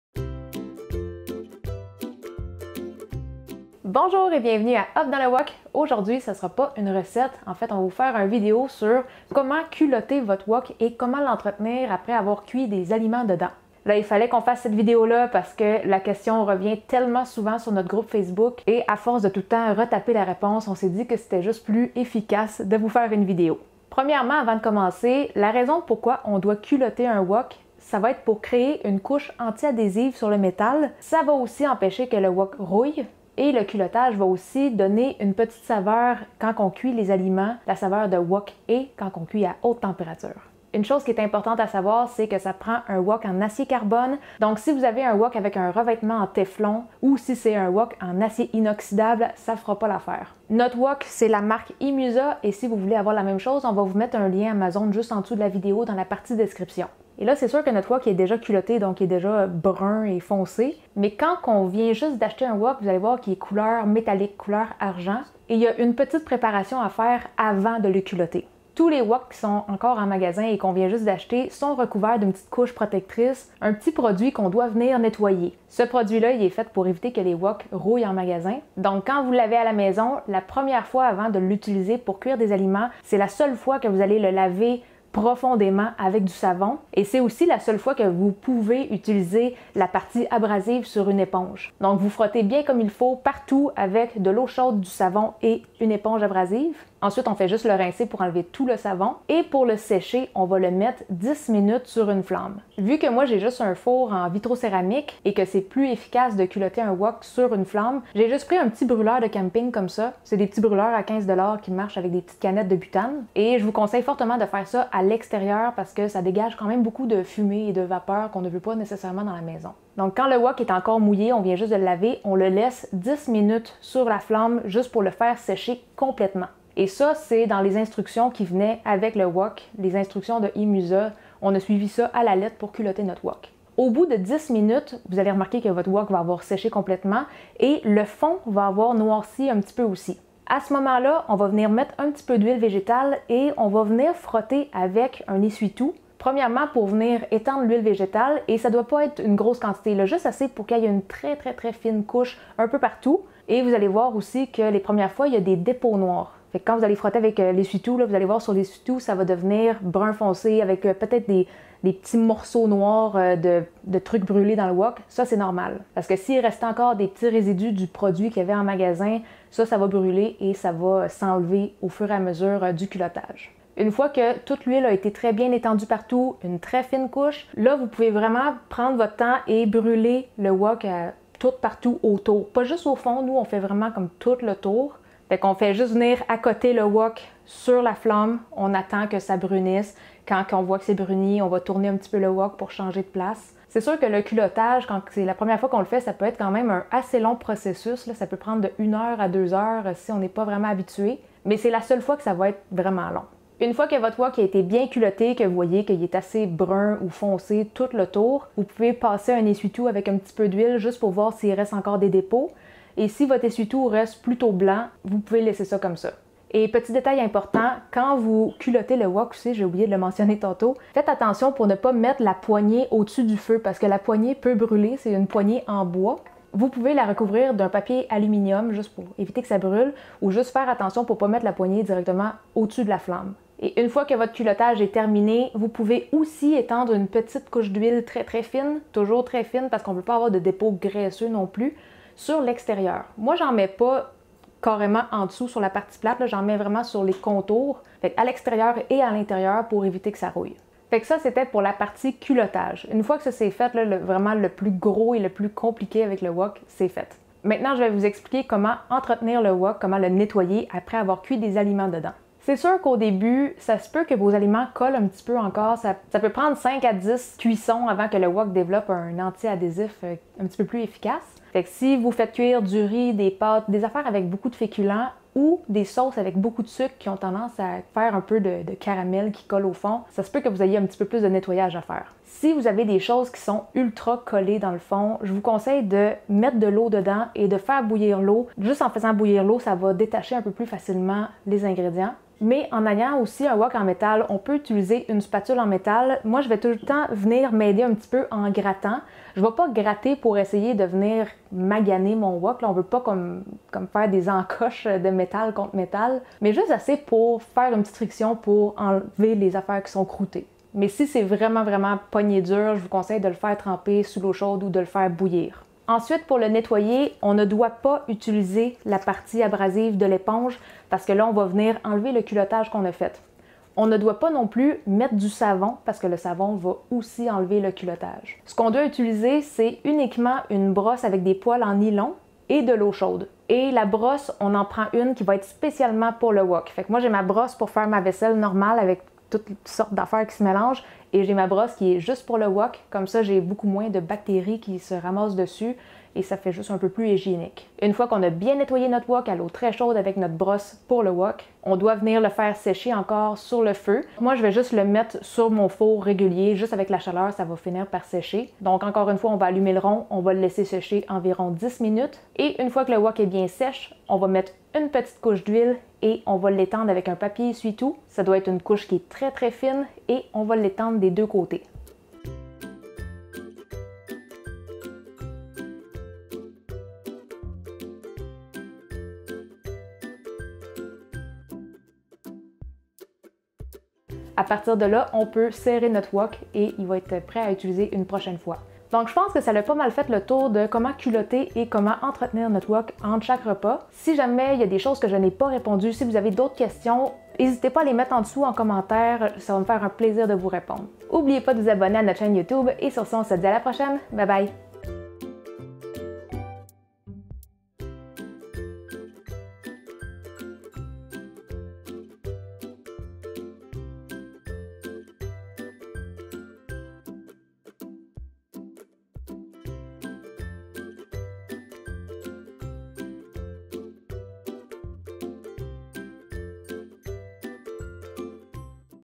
COMMENT CULOTTER ET LAVER UN WOK EN ACIER CARBONE - HOW TO SEASON AND CLEAN A WOK - HOP DANS LE WOK!
Dans cette vidéo, je vous explique comment j'ai culotté mon wok  en acier carbone, et aussi comment je l'entretiens entre chaque cuisson.

ARTICLES DE CUISINE UTILISÉS DANS LA VIDÉO
✦Wok Imusa en acier carbone
✦Brûleur au butane

ACCESSOIRES POUR TOURNAGE :
✦Kit de lumières Canadian Studio photo video
✦Grand Trépied